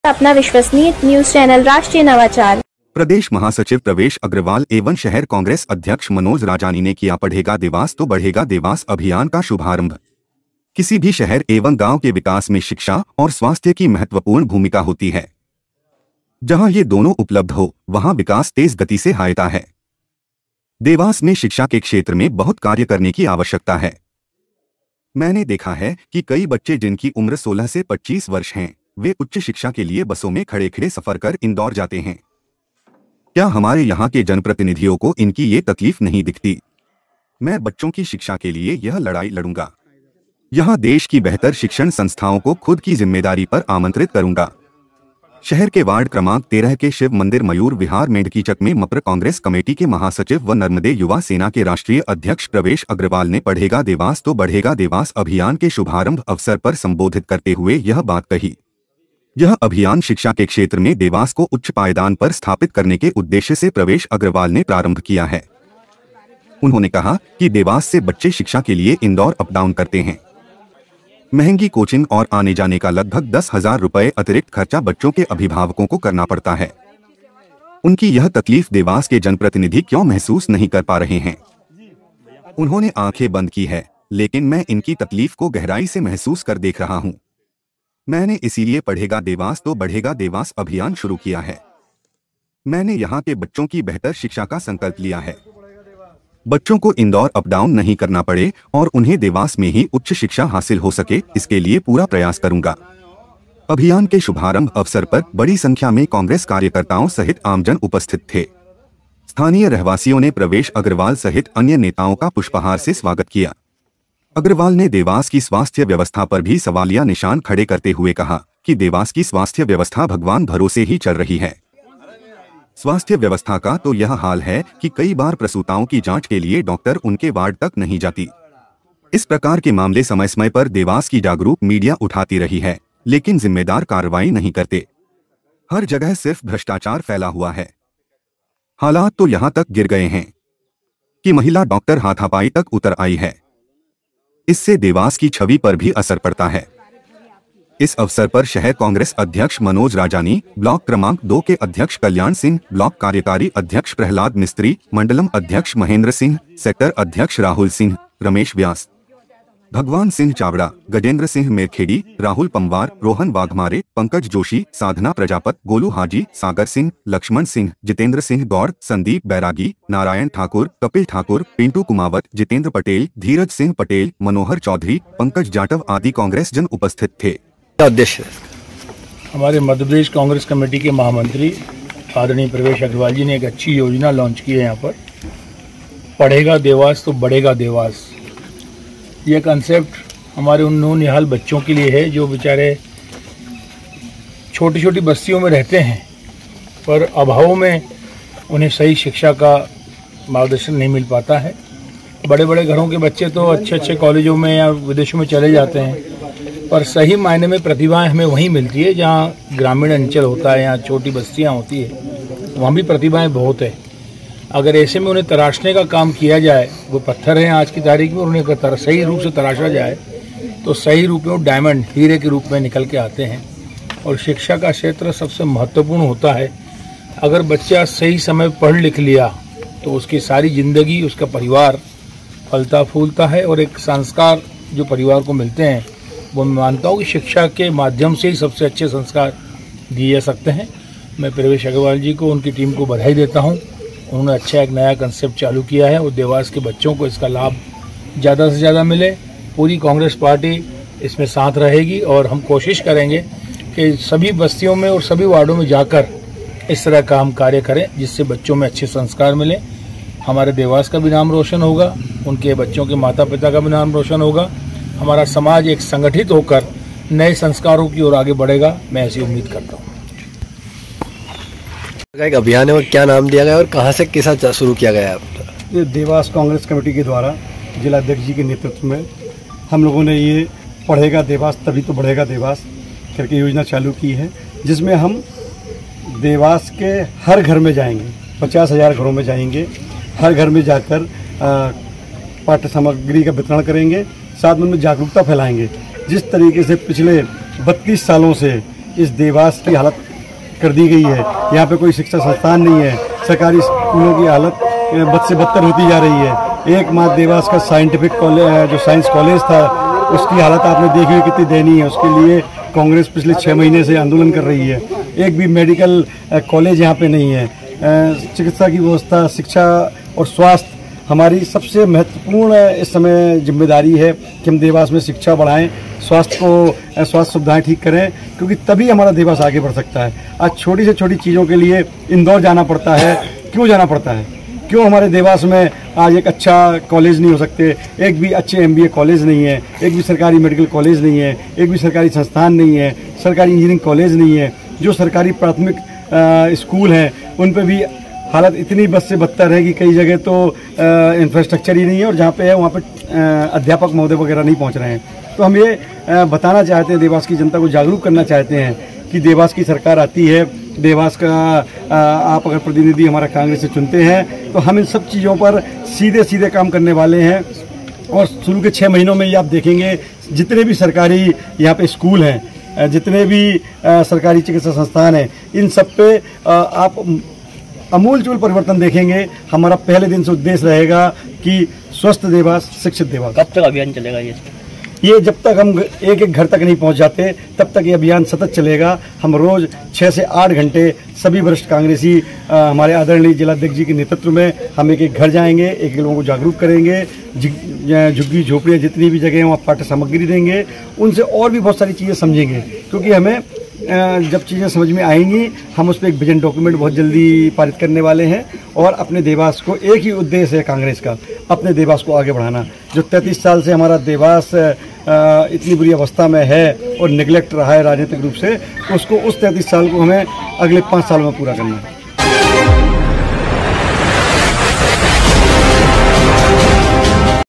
5साल में करके दिखाएंगे,जो 33साल से उपेक्षित देवास में नहींहुआ।कांग्रेस का स्वस्थ देवास,शिक्षित देवास
👉 *DRN : देवास में जो काम नहीं हुए वह 5 साल में करके दिखाएंगे। कांग्रेस का स्वस्थ देवास, शिक्षित देवास अभियान। देखिए क्या कहा शहर कांग्रेस अध्यक्ष मनोज राजानी और प्रदेश कांग्रेस महामंत्री प्रवेश अग्रवाल ने।*